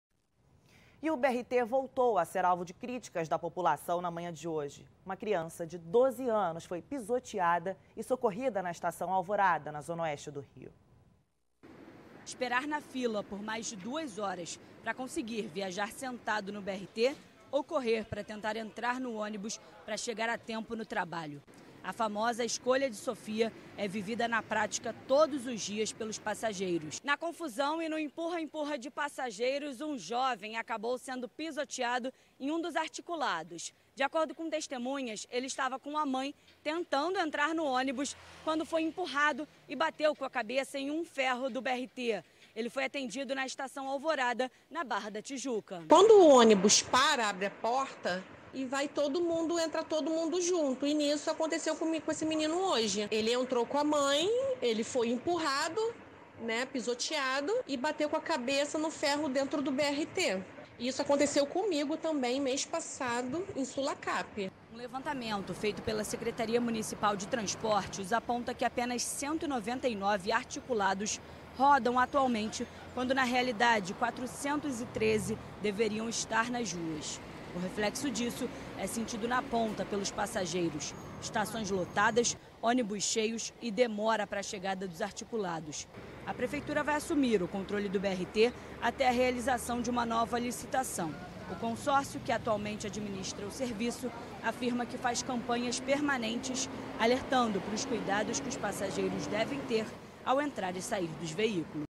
Criança de 12 anos é pisoteada em BRT na Barra
O BRT voltou a ser alvo de críticas de passageiros pela manhã.